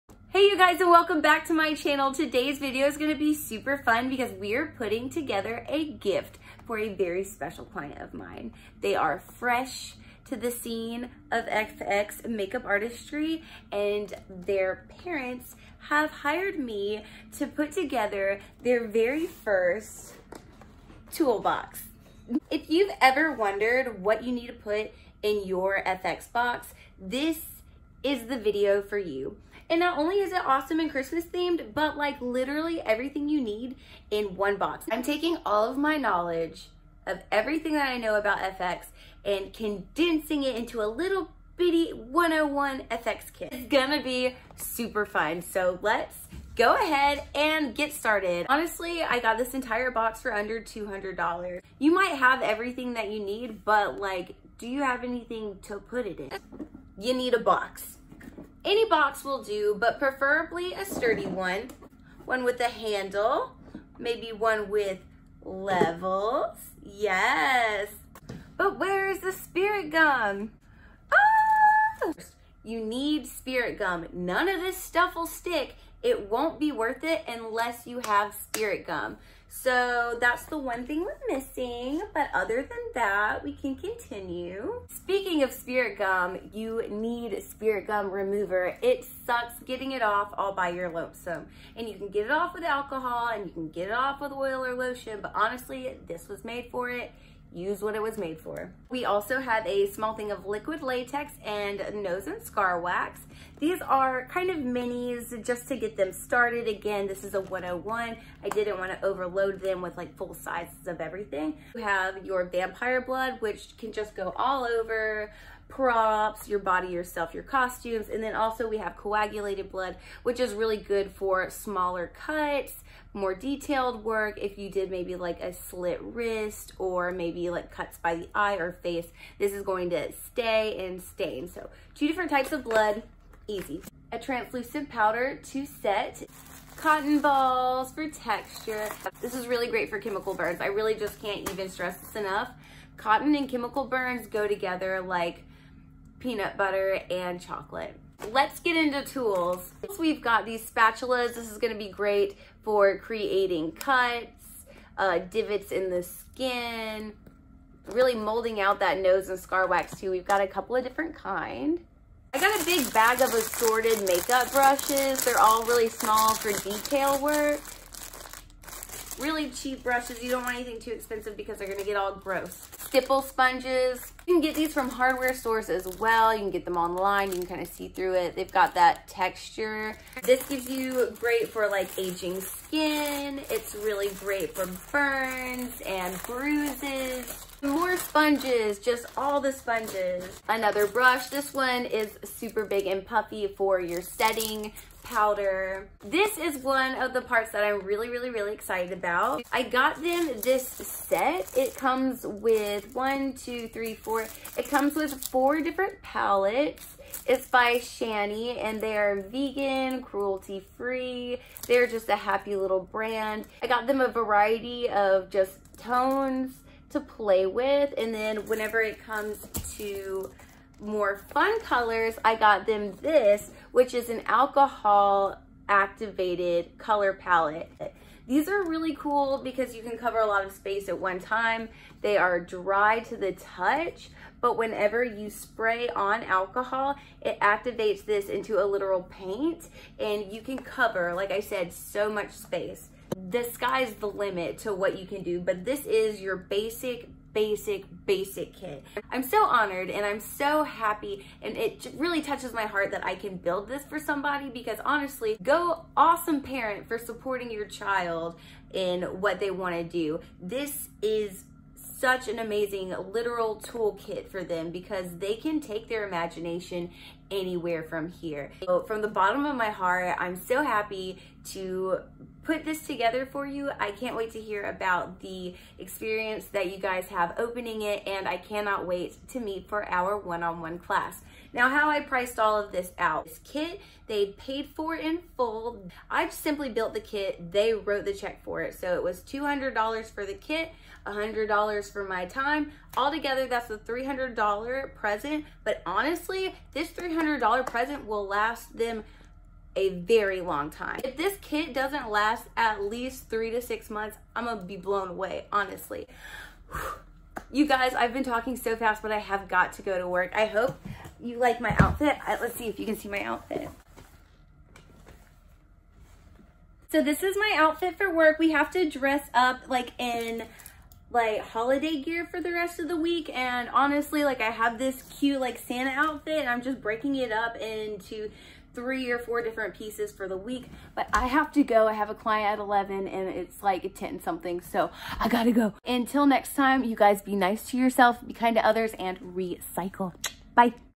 What to Put in Your Beginner FX Makeup Kit
Ever wonder what you need to build your very first FX Makeup Kit?! Here is my 101 guide on what all you need AND you can get this all on Amazon for under $200.
FOLLOW MY OTGHER SOCIALS
ProPage: @KissCosmeticss
PersonalPage: _anarchy_chaos_n_glitter